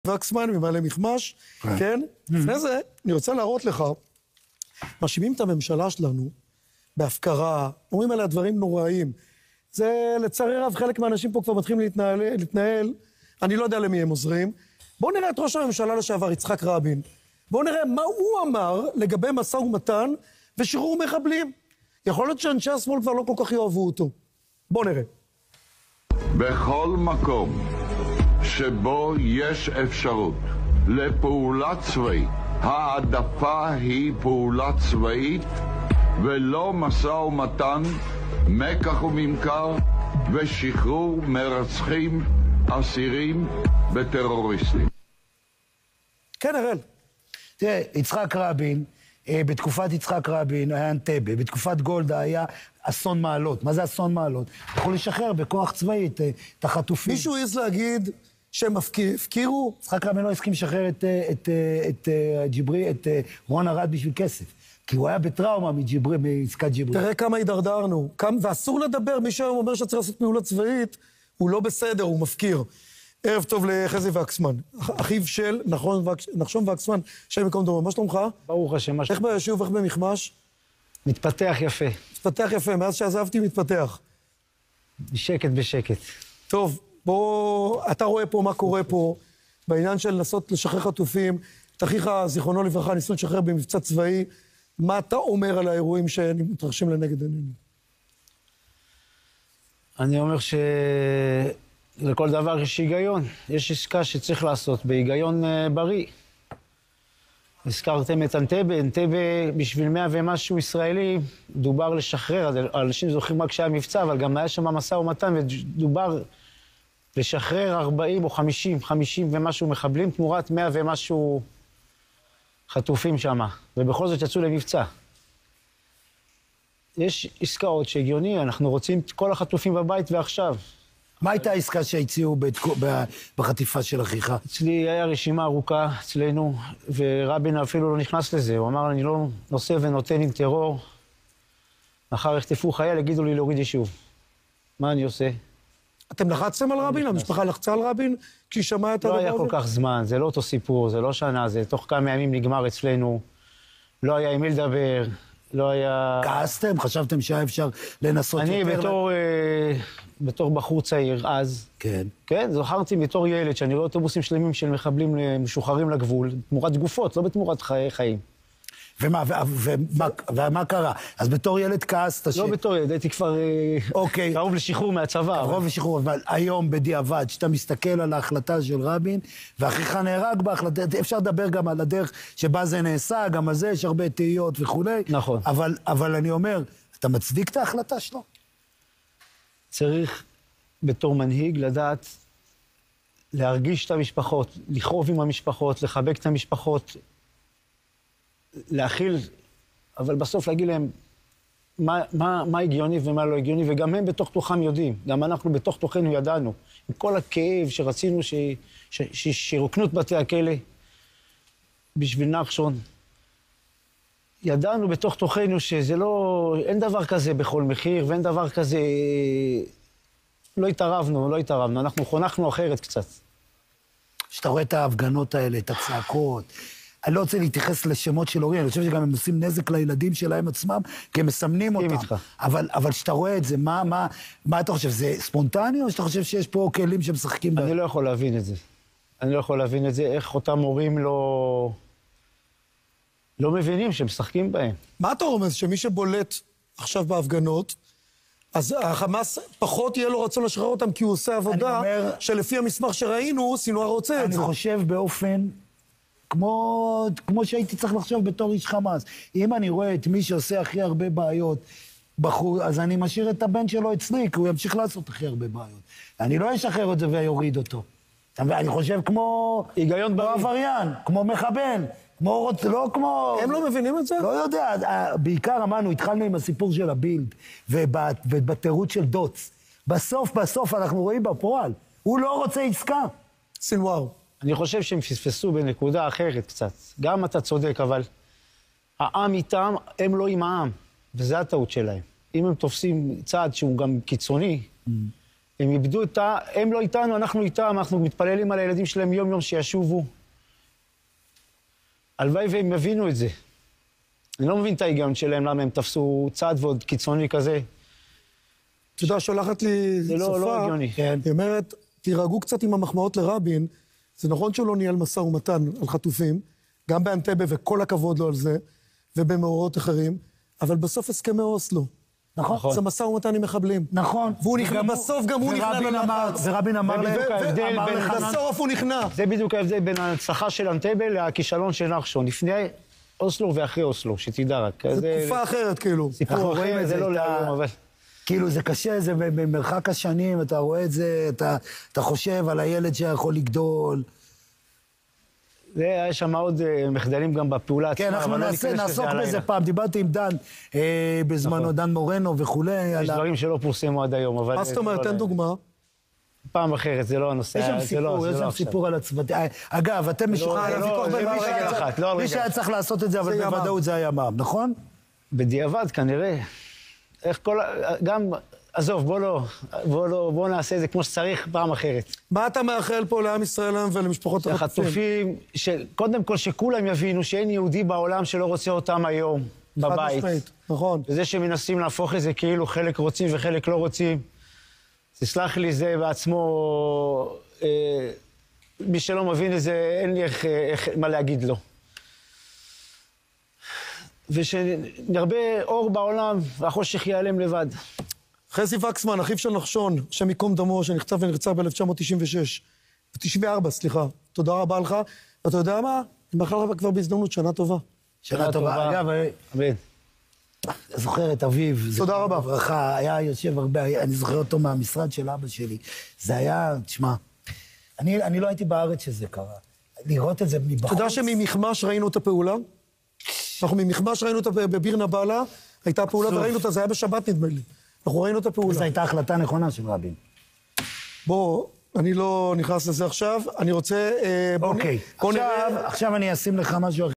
חזי ווקסמן: באנטבה דרשו לשחרר 50 טרוריסטים בשביל 100 חטופים ובכל זאת יצאו למבצע!!
חזי ווקסמן: כאשר הייתי צעיר יותר כעסתי, אבל עכשיו אני מבין שאין דבר כזה בכל מחיר!!

רבין: בכל חטיפה אסור להגיע לעסקה אלה יש לפעול באופן צבאי